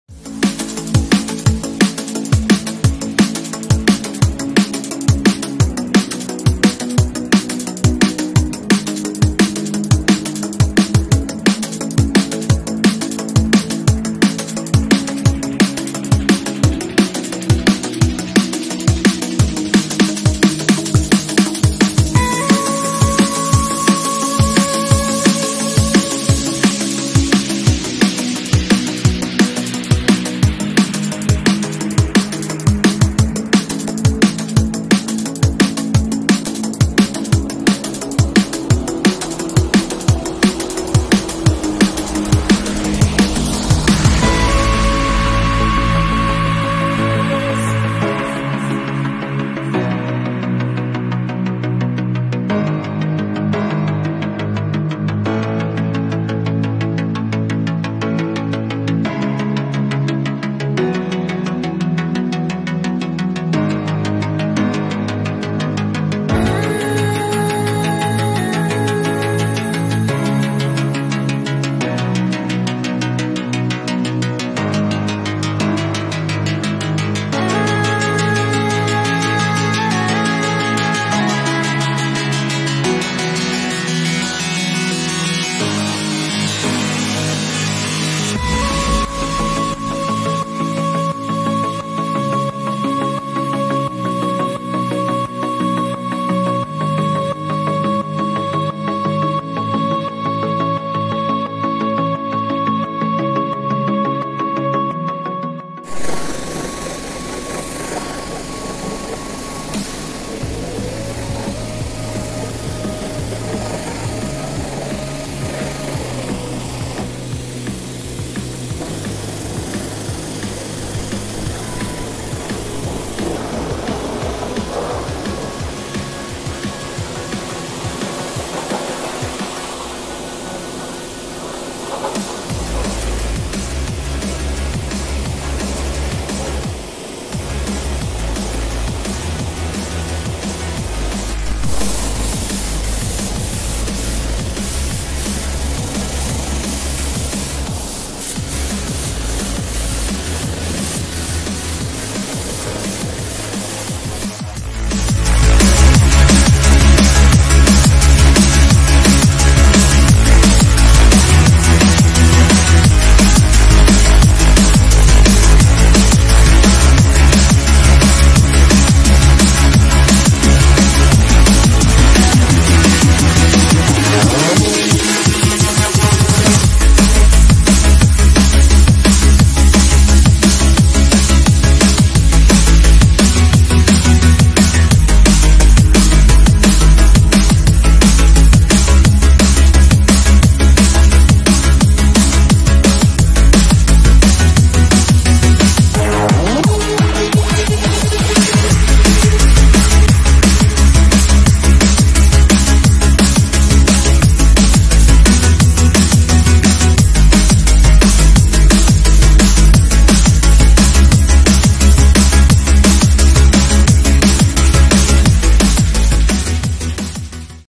lavado y engrasado a vapor Jeep Cherokee
lavado y engrasado incluye : lavado de motor a vapor, chasis y Suspensión a presión, engrasado de suspensión con grasa sintética lubricacion de chasis y tolvas revisión de frenos y suspensión así como puntos de seguridad, lavado de carrocería y aspirado